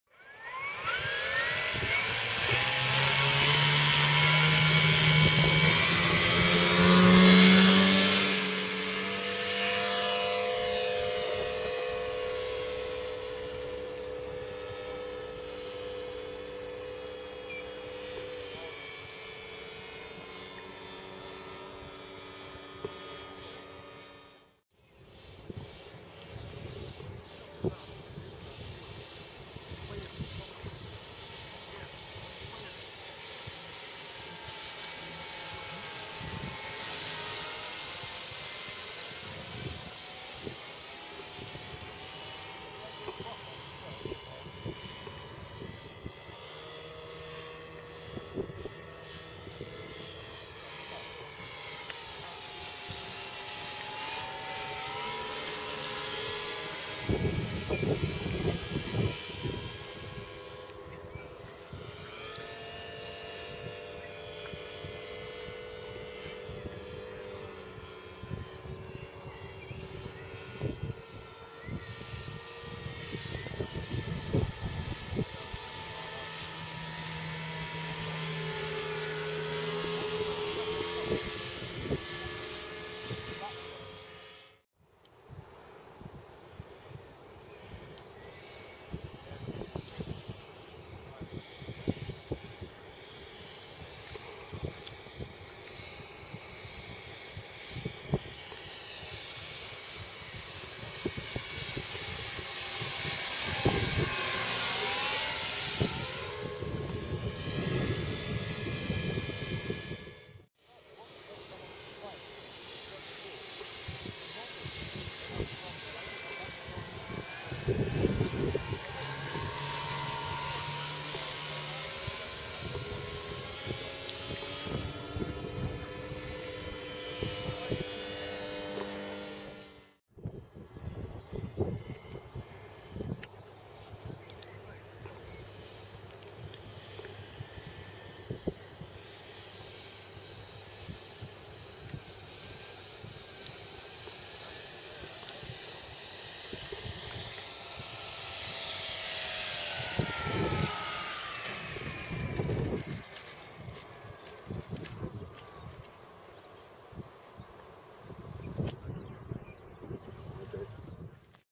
Peterf's 1/4 scale Tiger Moth video 2
The eigth flight of my quarter scale Tiger Moth from the Falcon Models Kit. Wingpsan is 88" (2.25m) and the total flying weight of the plane with batteries is 18lb 1oz (8.2kg).